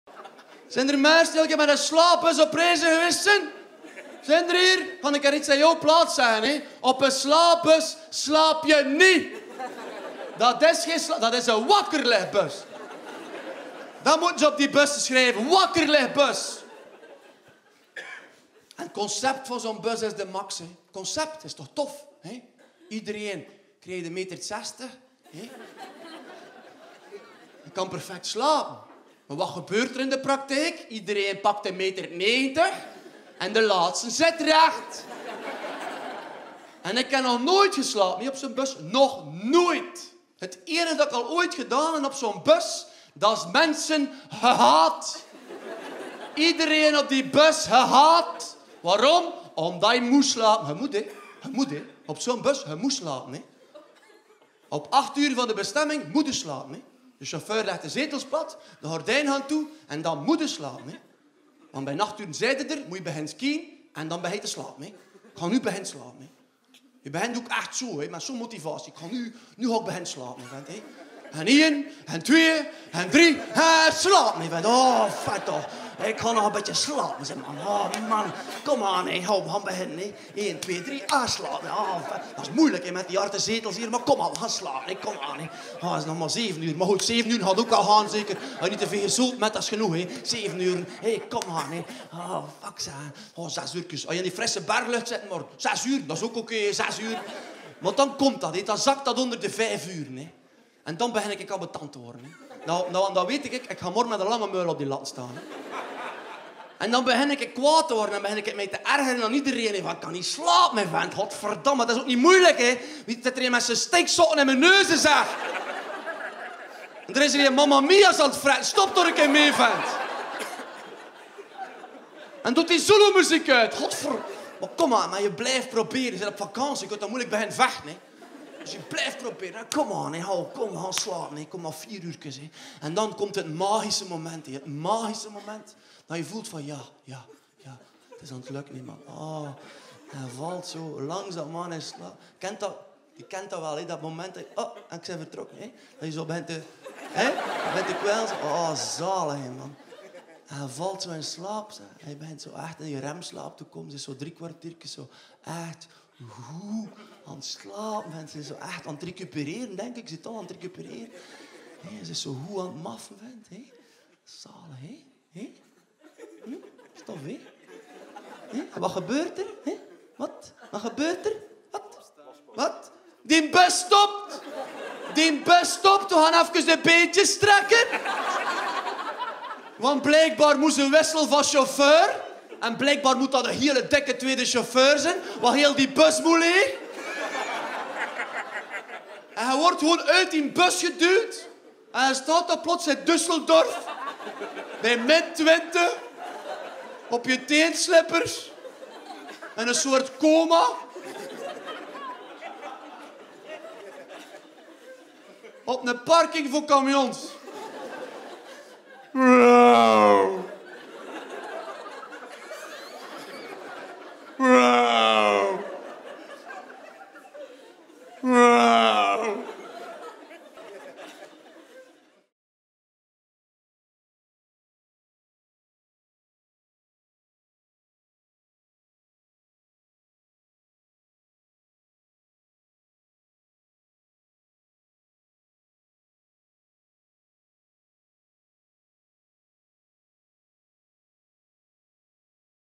Steven Mahieu over de slaapbus
Uit de eerste theatershow 'Mahieustueus'.